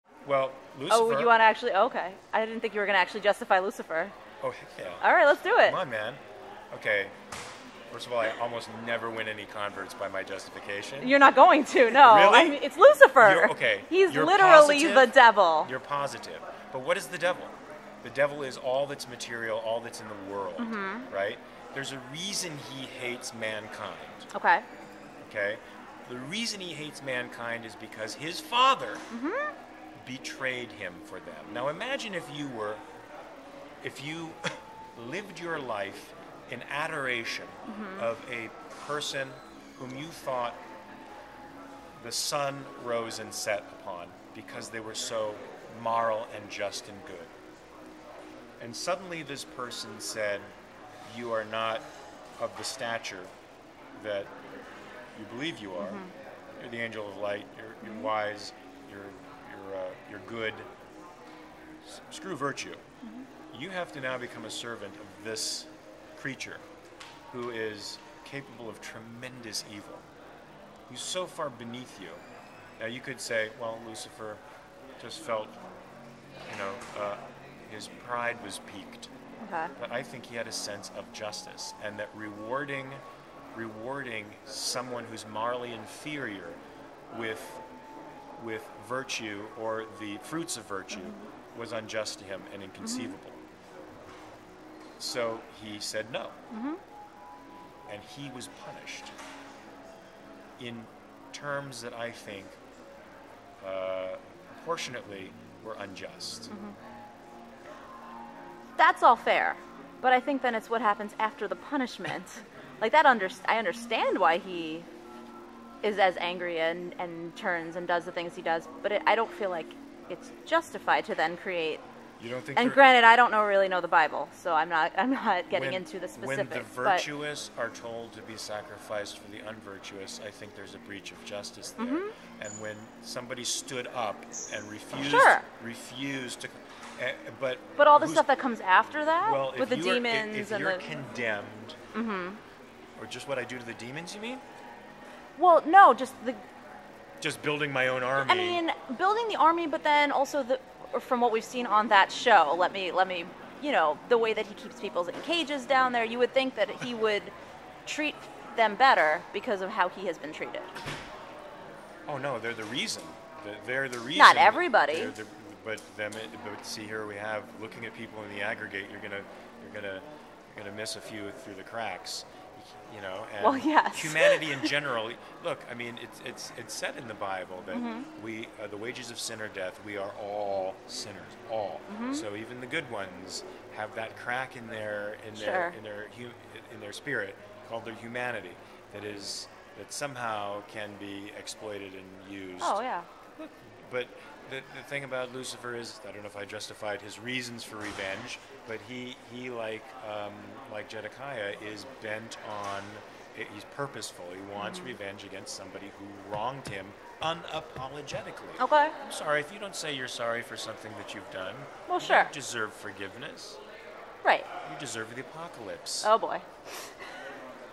Mark Pellegrino tries to justify Lucifer's reasons on 'Supernatural'
I sat down with Mark Pellegrino to talk about his new CW series 'The Tomorrow People' but we ended up on a tangent about 'Supernatural', which led to his attempt to justify Lucifer's reasoning. I see his point, but I still think Lucifer was way harsh, Tai!